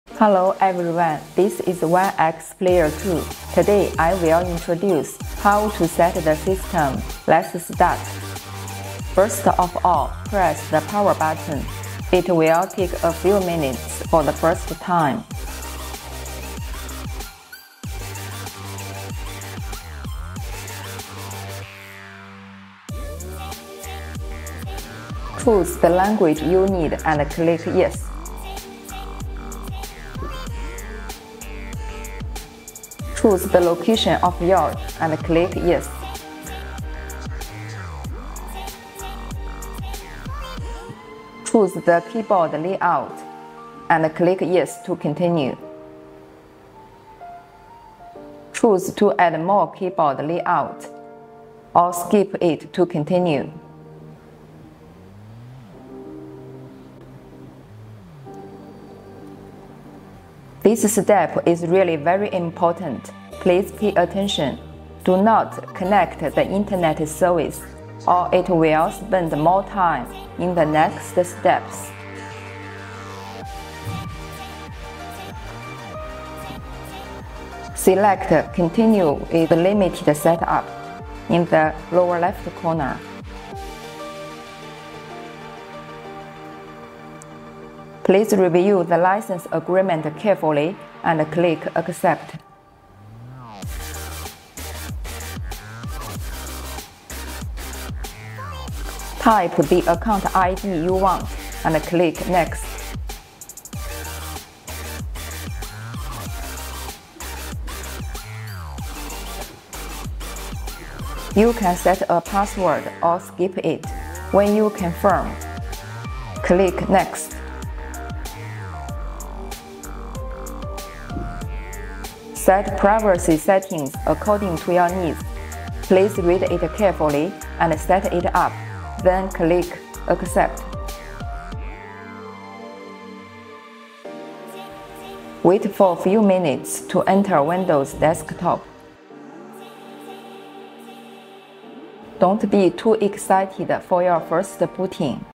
OneXPlayer 2: System Setting
First of all, press the power button. It will take a few minutes for the first time.
Step 1 Choose the language you need, and click “Yes”.
Step 2 Choose the location of yours. Click “Yes”.
Step 3 Choose the keyboard layout. Click “Yes” to continue.
Step 4 Choose to add more keyboard layout, or skip it to continue.
Step 5 This step is really very important. Please pay attention! Do not connect the Internet service, or it will spend more time in the next steps.
Step 6 Select “Continue with limited setup” in the lower left corner.
Step 7 Please review the license agreement carefully, and click “Accept”.
Step 8  Type the account ID you want, and click “Next”.
Step 9 You can set a password or skip it. When you confirm, click “Next”.
Step 10 Set privacy settings according to your needs. Please read it carefully and set it up. Then click “Accept”.
Step 11 Wait for few minutes to enter Windows desktop.